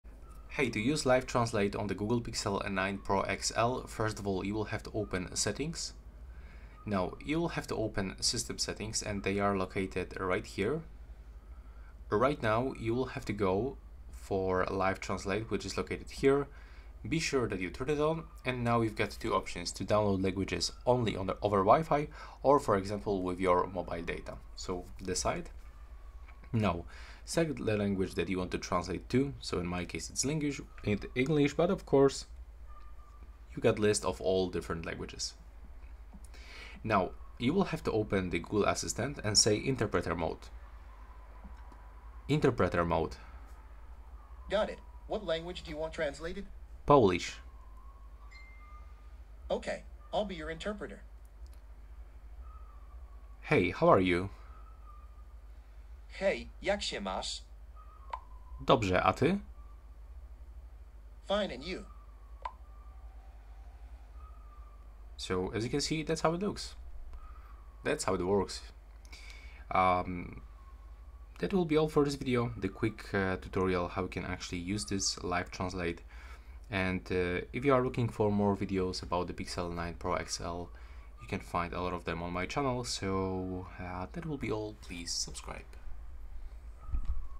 Pixel 9 Pro XL: How to Use Live Translate
Learn how to use Live Translate on your Pixel 9 Pro XL with this easy guide. This video will show you the steps to activate and utilize the Live Translate feature on your Google Pixel 9 Pro XL, enabling real-time translation of text and speech. Discover how to break language barriers effortlessly with this powerful tool. Perfect for anyone looking to communicate across languages using their Pixel 9 Pro XL.